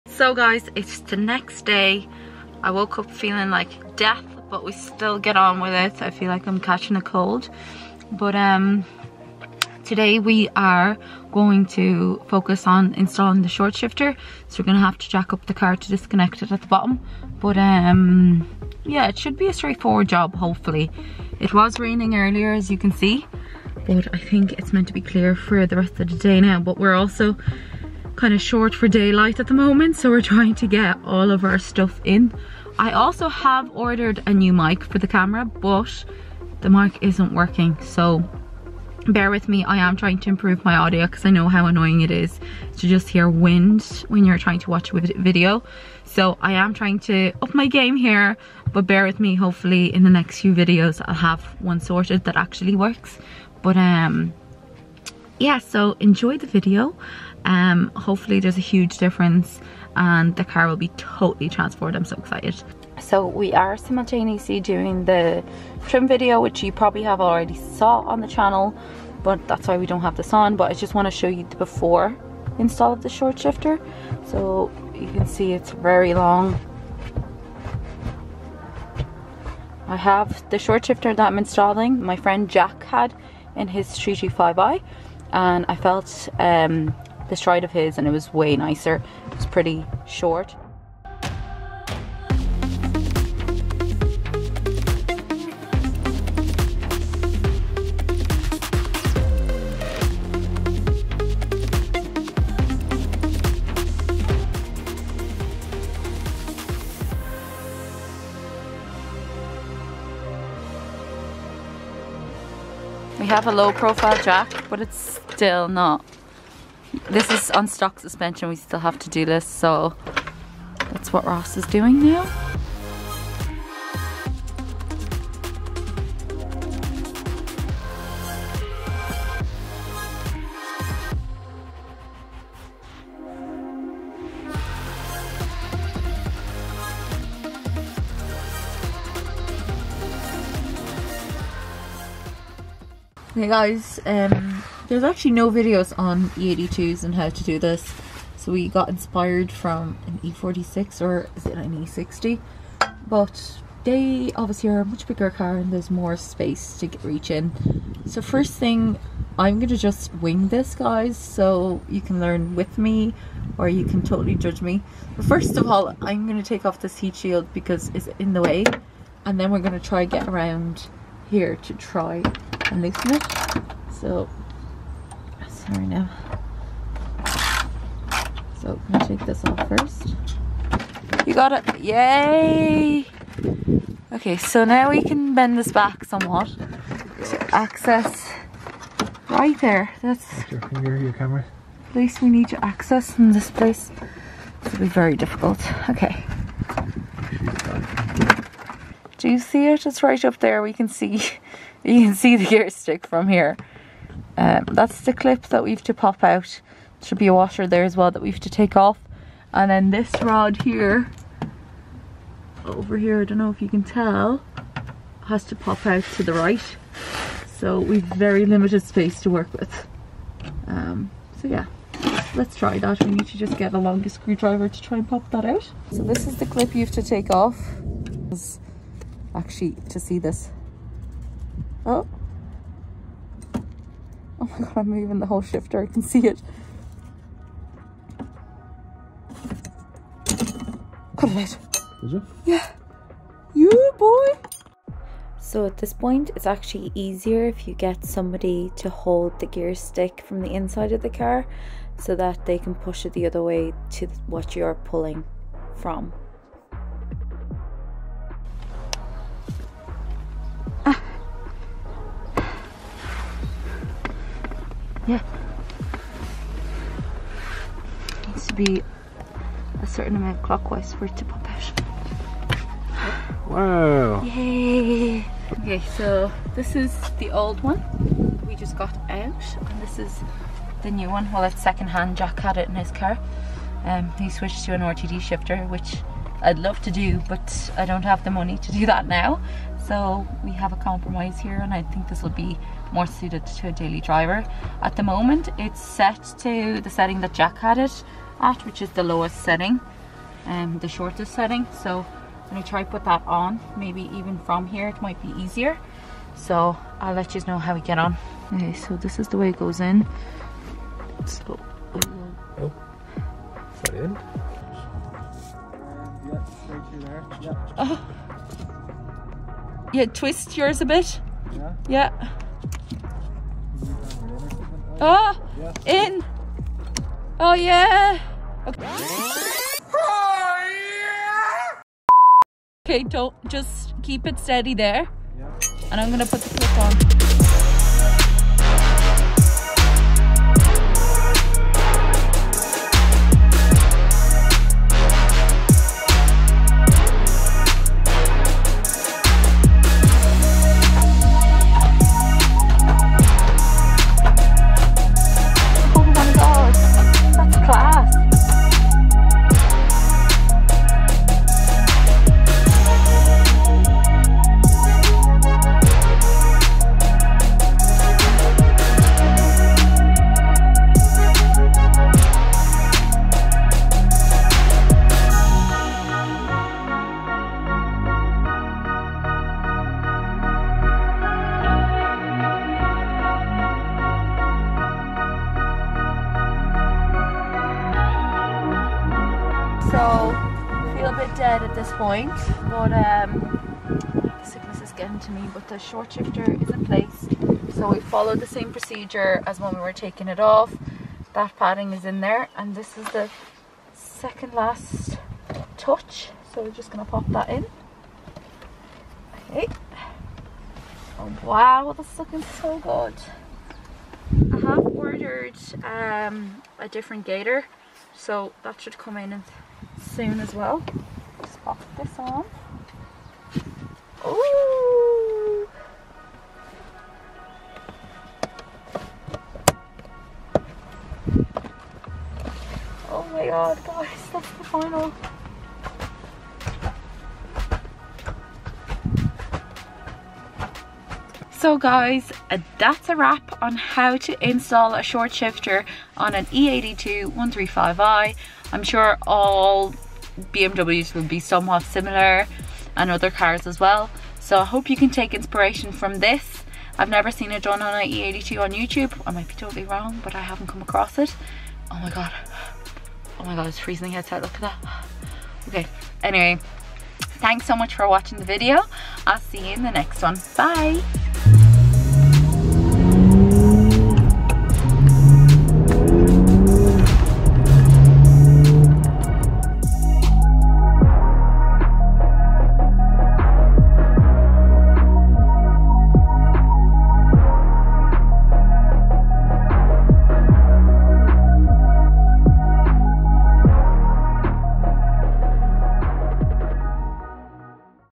135i gets a SHORT SHIFTER | Schmiedmann Short Shifter Install
Hi Guys!

In this video I am showing you how I install the Schmiedmann shortshifter on my 135i. For once it's actually somewhat straightforward.

Monika x

Editor: @rossk3lly